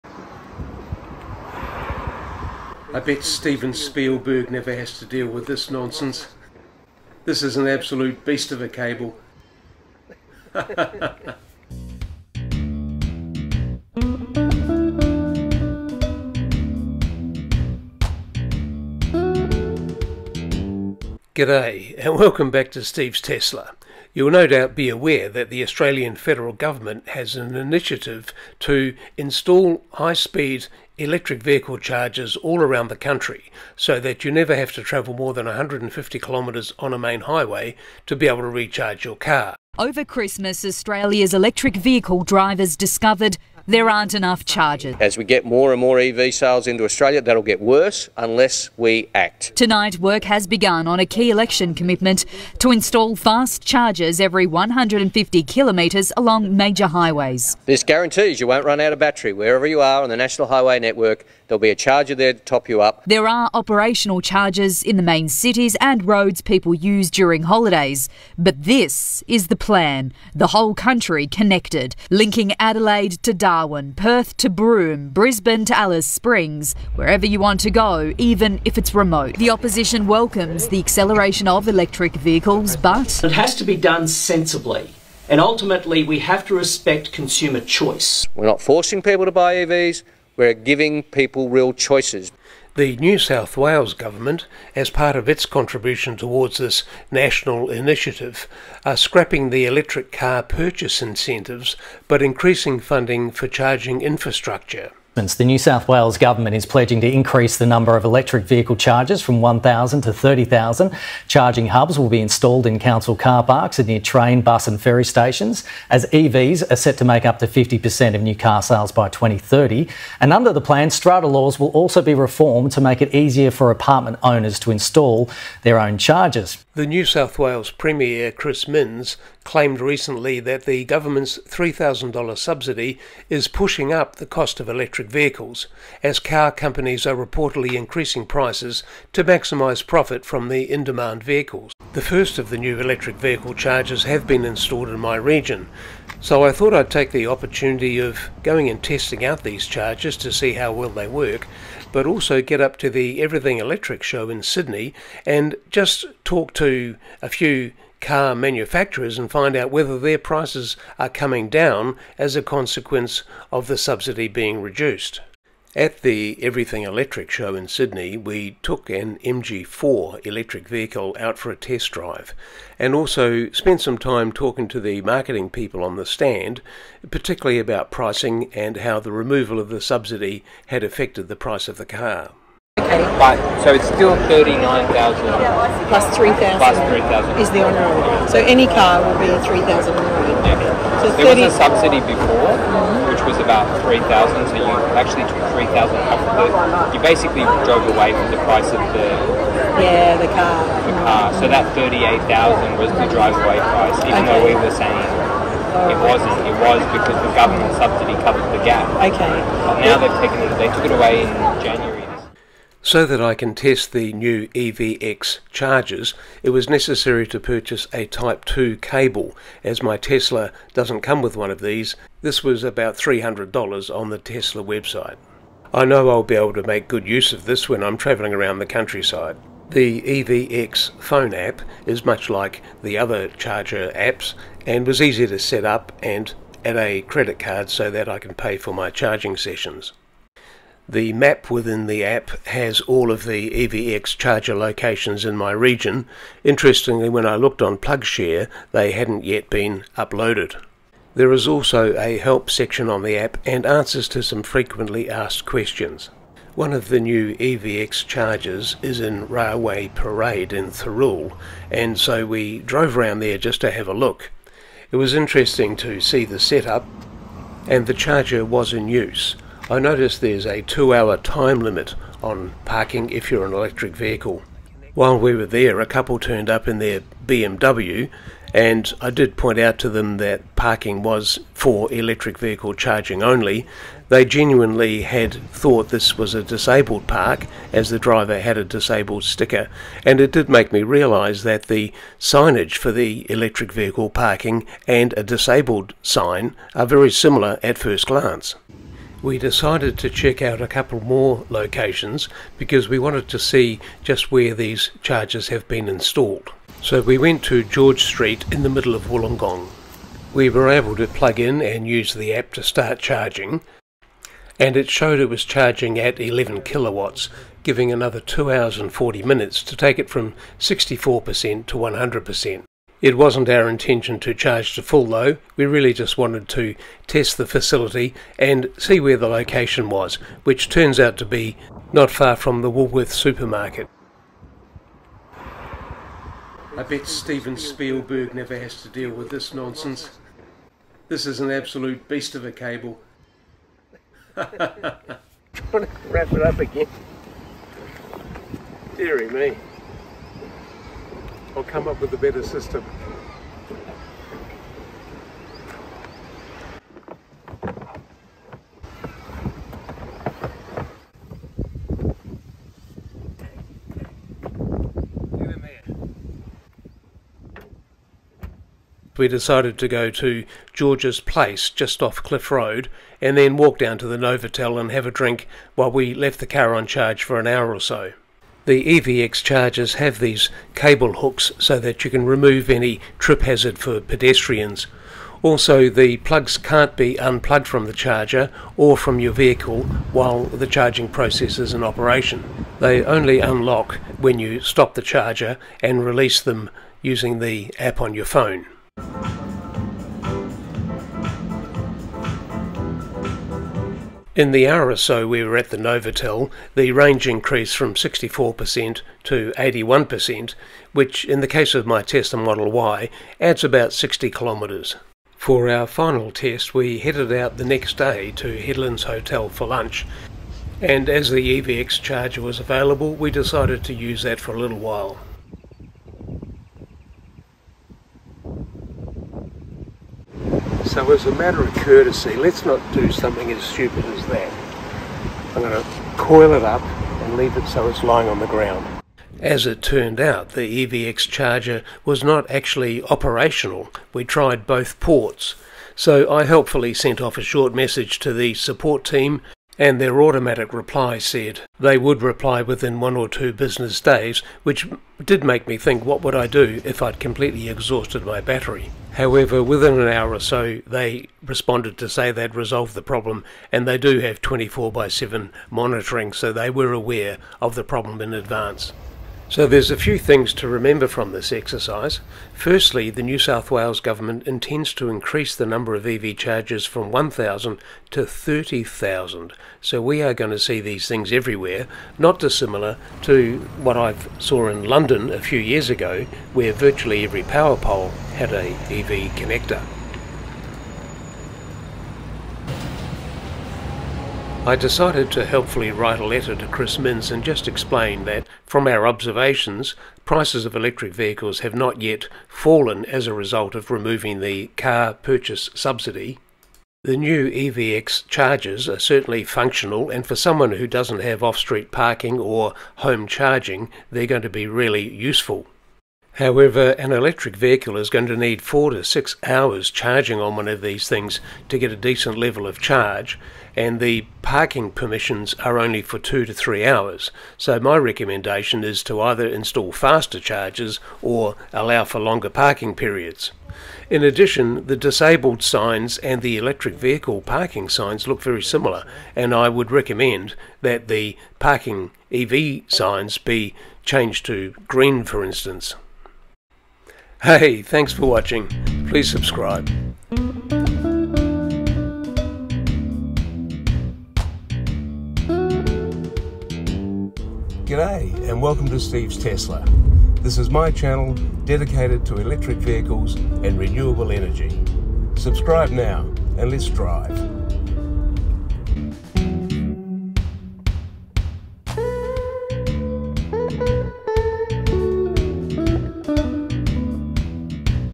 30,000 EV chargers for NSW
The NSW Government has recently removed the EV purchase subsidy and is spending the money on EV charging infrastructure. We investigate if new EV prices are coming down, and also how effective the new EVX chargers are in real life.